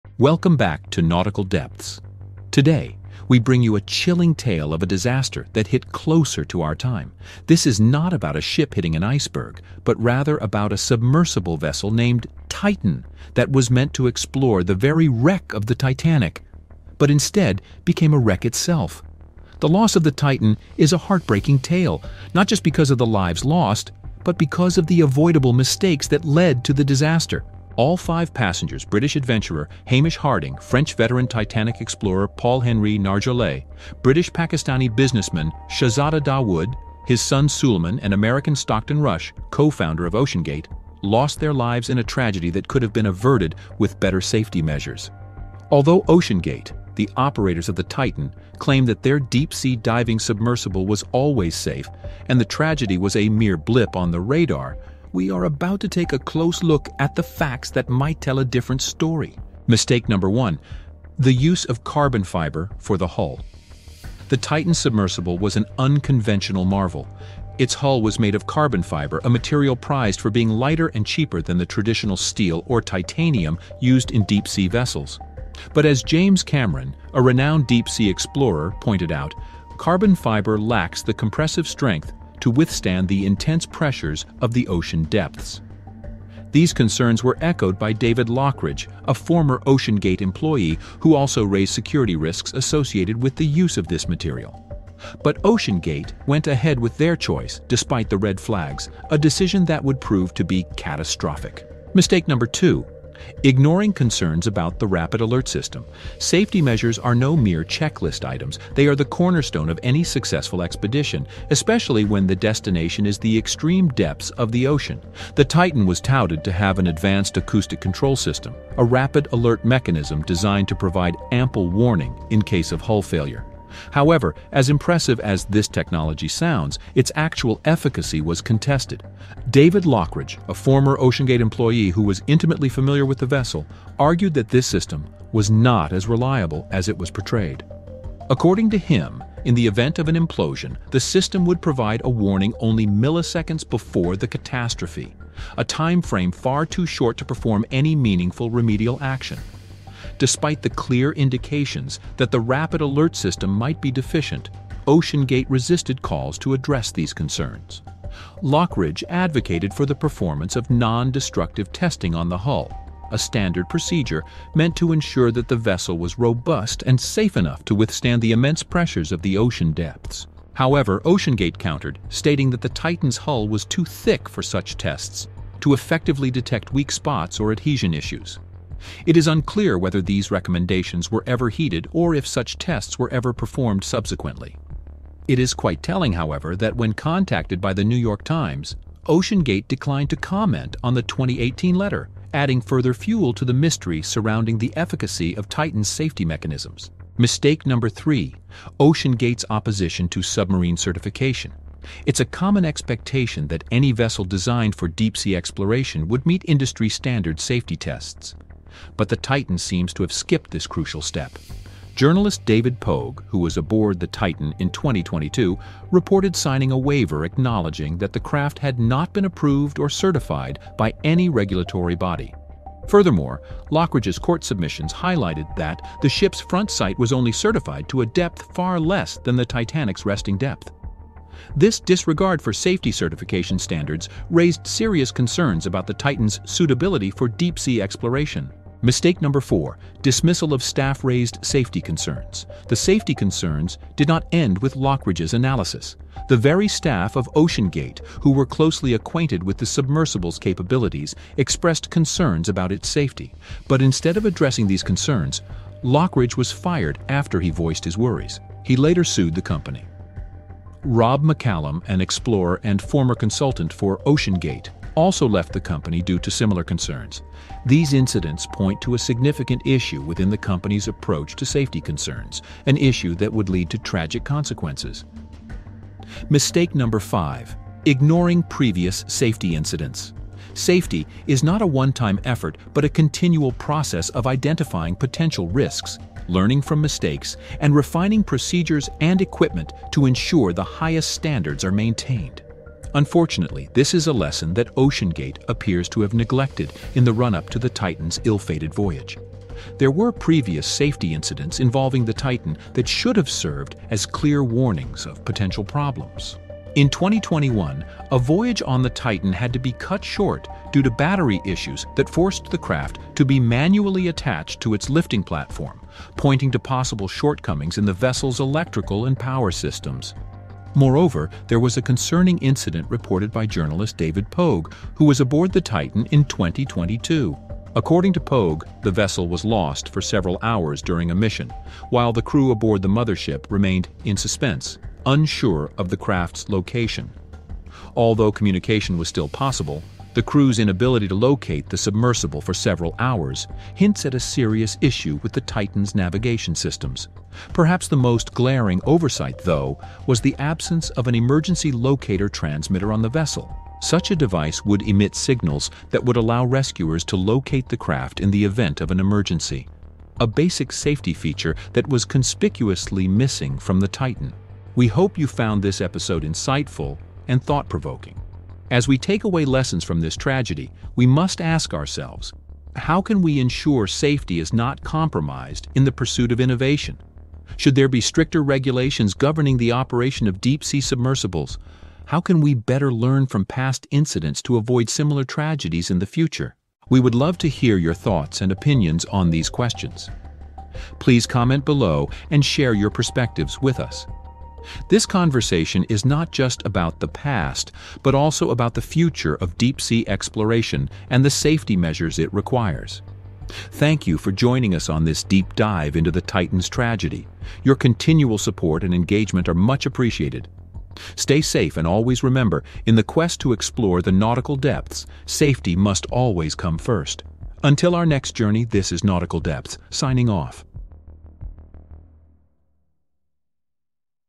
Shocking! 5 Avoidable Blunders That Led to Oceangate Titan Tragedy!
Join us on Nautical Depths as we delve into the tragic tale of the OceanGate Titan, a submersible vessel that met a disastrous end due to avoidable blunders. We'll explore the five critical mistakes that led to this heartbreaking incident, from the use of carbon fiber for the hull to the dismissal of staff-raised safety concerns. This video is not just a recounting of the past, but a call to action for the future of deep-sea exploration. Learn from the Titan tragedy and join the conversation on how we can prioritize safety in the pursuit of innovation.